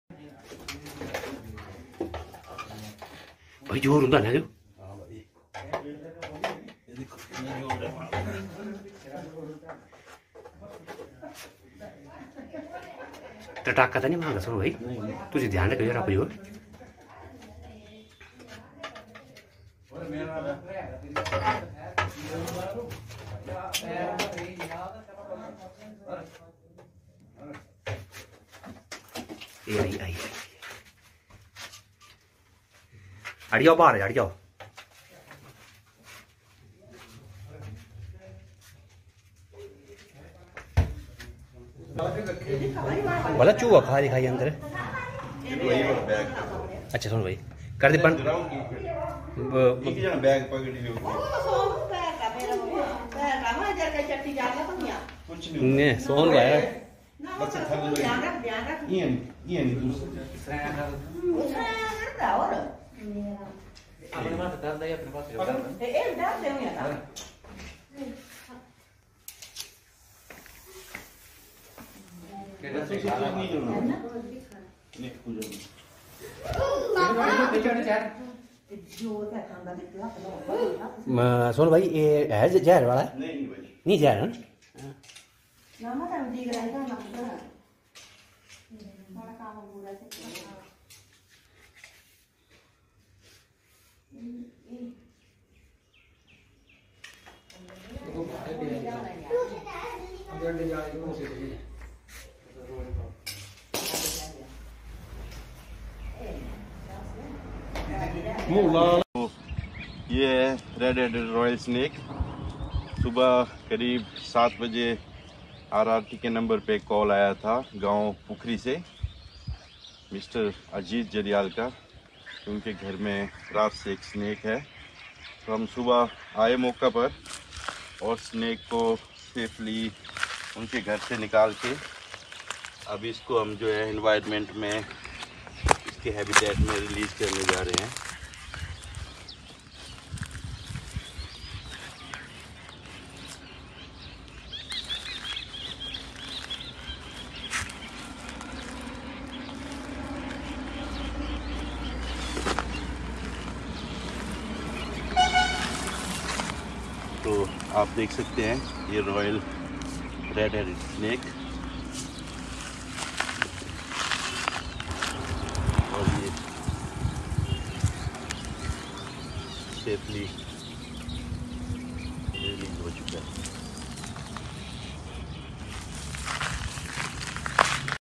चंबा में एक घर से निकला 7 फीट लंबा रॉयल Snake | Himachal wildlife | Snake rescue | Hindi News
The RRT of the Forest Department Chamba Division has rescued a Royal Snake from a house in Pukhri village. After catching this nearly seven-foot long Royal Snake, the forest department team has left it safely in the forest.

वन विभाग चम्बा  मंडल की आरआरटी ने पुखरी गांव में एक मकान से रॉयल स्नेक को रेस्क्यू किया है। करीब सात फुट लंबे इस रॉयल स्नेक को पकड़ने के बाद वन विभाग की टीम ने सुरक्षित जंगल में छोड़ दिया है।